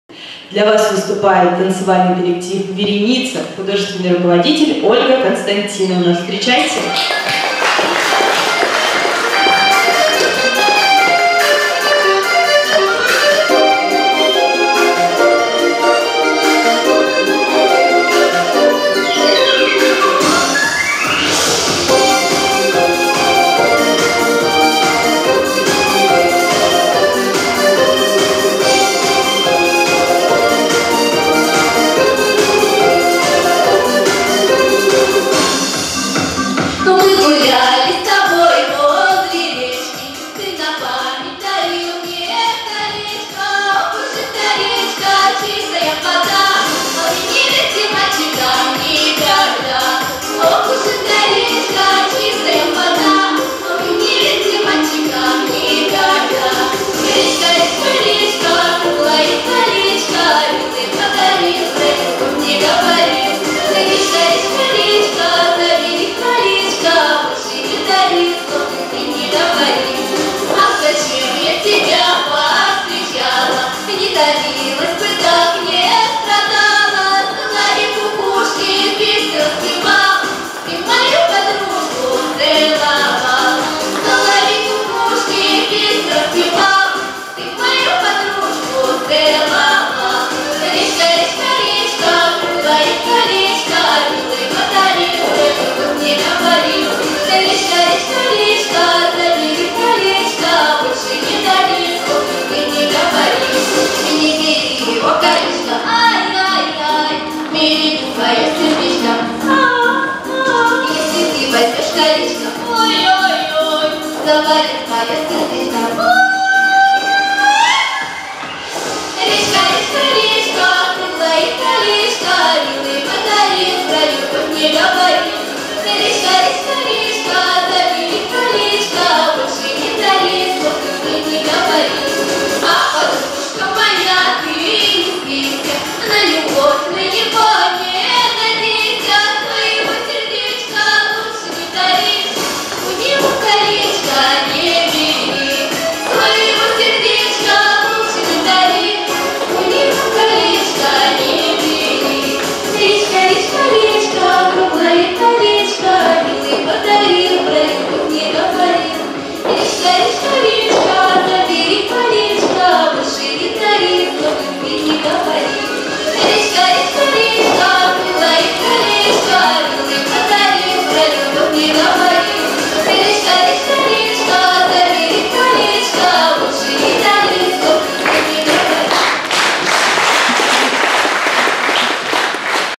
Танцевальный коллектив "Вереница". Праздничный концерт "День мудрости и добра".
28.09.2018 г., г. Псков